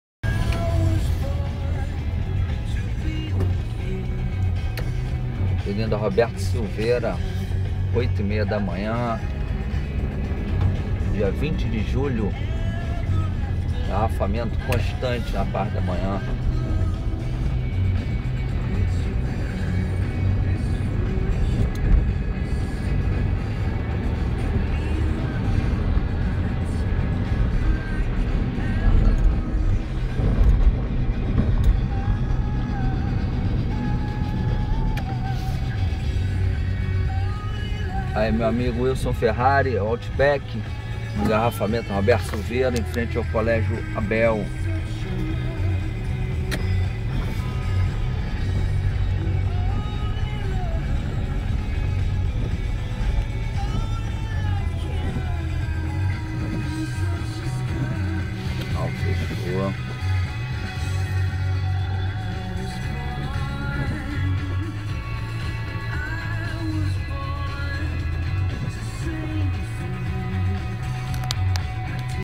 Empenas Av Roberto Silveria 132 e 18 mp4
Empenas Av Roberto Silveria 132 e 18
Transito engarrafado em dia de chuva